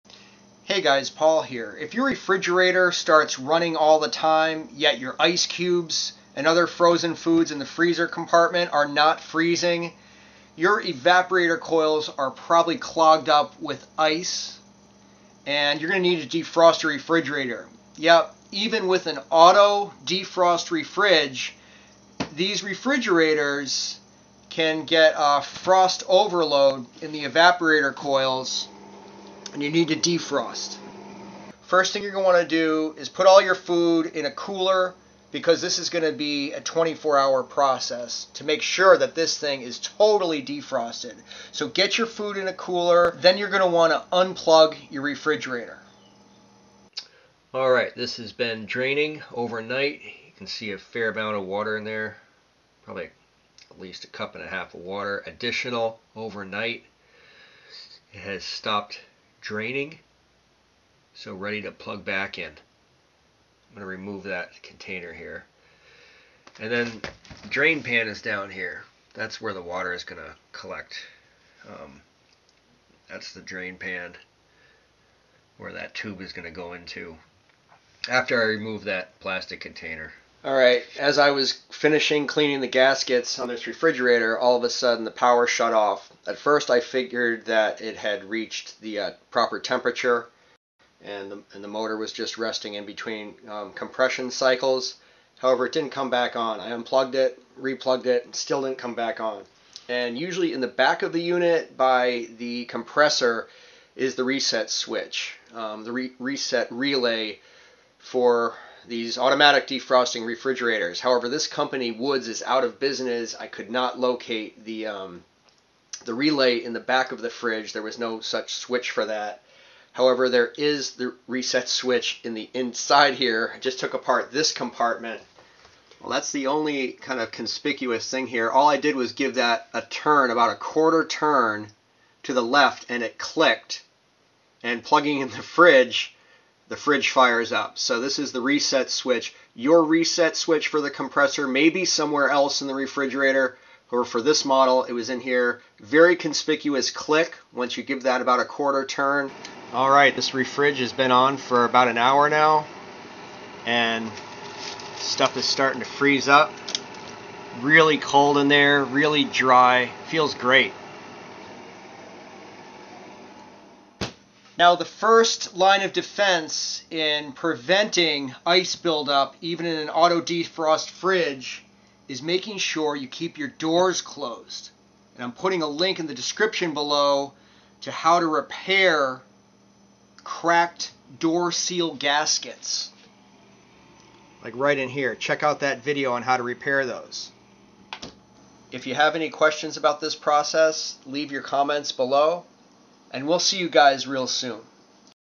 Fix Freezer Ice Build-Up: Reset Defrost Timer Switch
*Unplug refrigerator before touching any electrical wires.* When freezer door doesn't close, ice builds up on evaporator coils, and fridge will run constantly, while leaving food unfrozen. It's time to defrost. Even auto-defrost fridges will build up ice over time.

If your refrigerator does not start, turn the reset relay switch 1/4 turn till it clicks. (This may also be a defrost timer switch.) Switch is located in back of fridge near compressor, or interior of fridge. Unscrew compartment and search for switch.